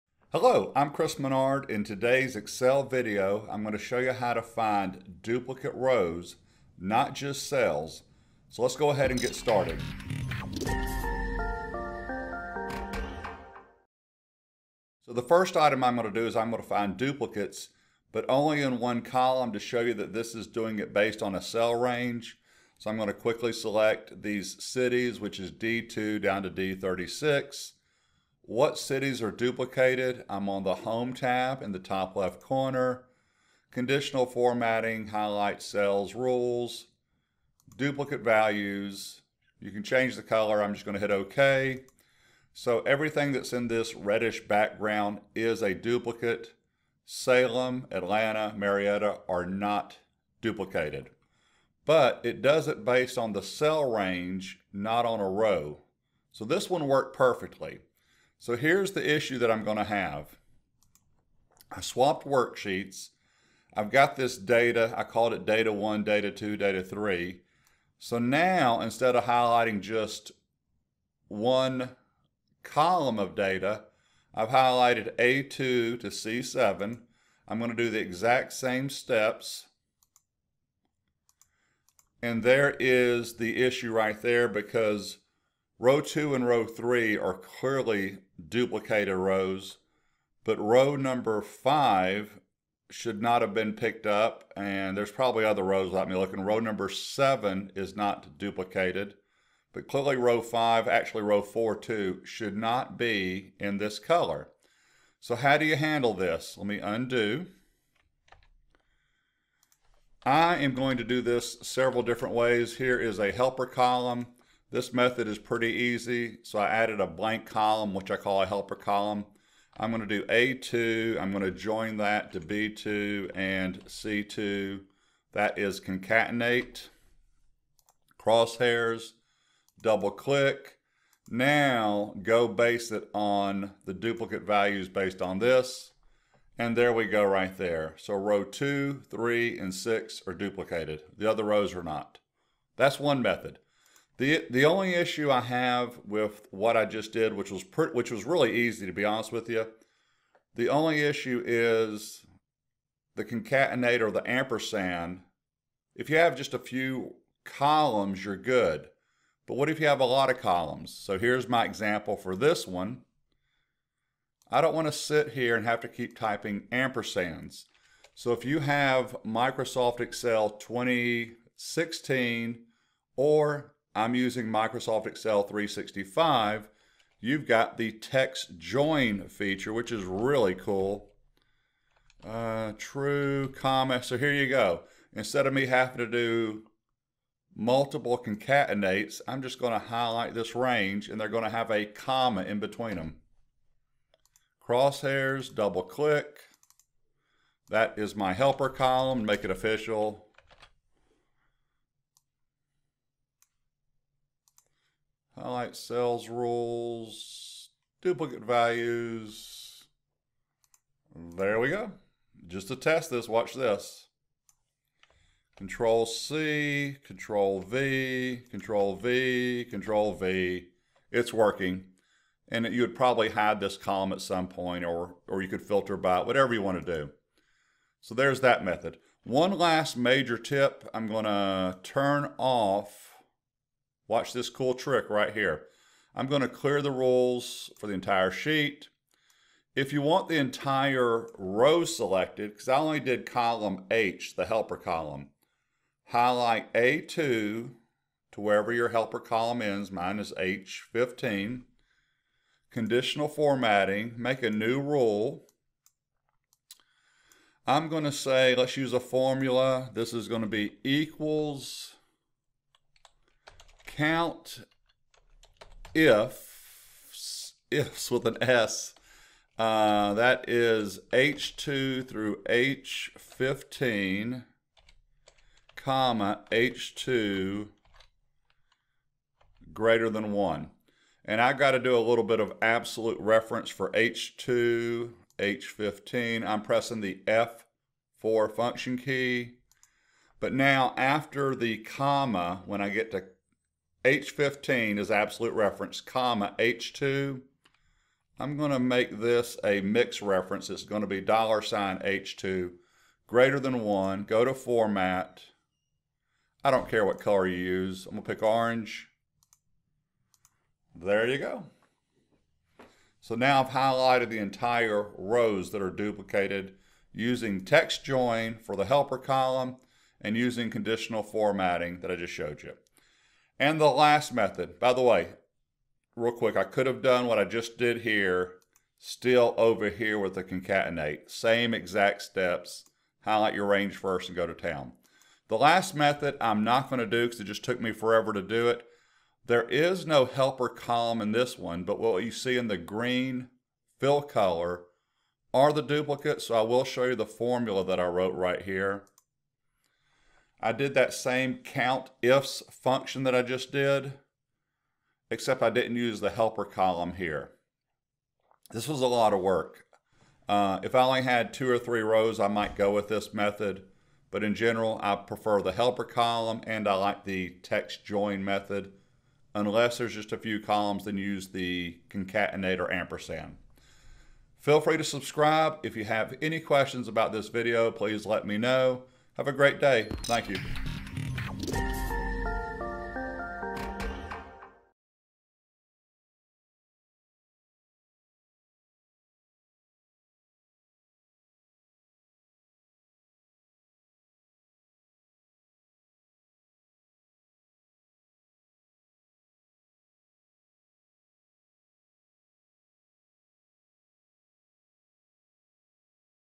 Excel - Find & Highlight Duplicate Rows - 3 Methods | Conditional Formatting
Microsoft Excel can find duplicates easily with Conditional Formatting. The issue is Conditional Formatting finds duplicates based on the cell value, but I want to find duplicate rows. I use three methods in the video to find duplicate rows. I start off with CONCATENATE, then use the TEXTJOIN function, and finally COUNTIFS with no helper column. To highlight the rows I use Conditional Formatting using a formula.

Chapters:
00:00 Introduction
00:14 Duplicate cells
00:58 Conditional formatting works on cells
01:49 Method 1 - Concatenate
02:39 Method 2 - TEXTJOIN
03:47 Conditional formatting entire row
05:23 Method 3 - COUNTIFS with no helper column
06:14 Closing remarks

CONCATENTATE in Excel
Use CONCATENATE, one of the text functions, to join two or more text strings into one string. You can use an ampersand also. Example: cell A2 has Chris and cell B2 contains Menard. =a2&" "&B2 will return Chris Menard in cells C2.  Same for row 3. In cell C3, could use =CONCATENTATE(A3," ",B3) to get Chris Menard

-- EQUIPMENT USED
○ My camera